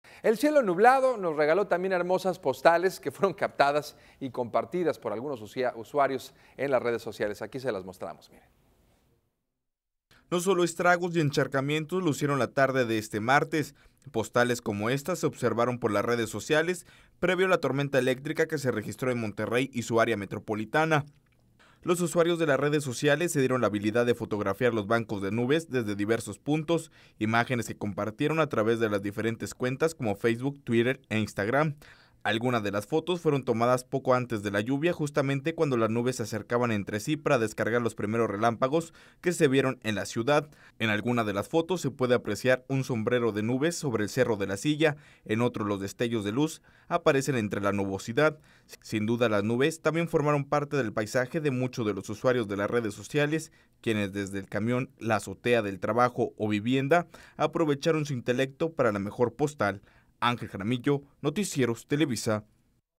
Las Noticias - Retratan paisajes de nubes
Los usuarios de las redes sociales se dieron la habilidad de fotografiar los bancos de nubes desde diversos puntos, imágenes que compartieron a través de las diferentes cuentas como Facebook, Twitter e Instagram.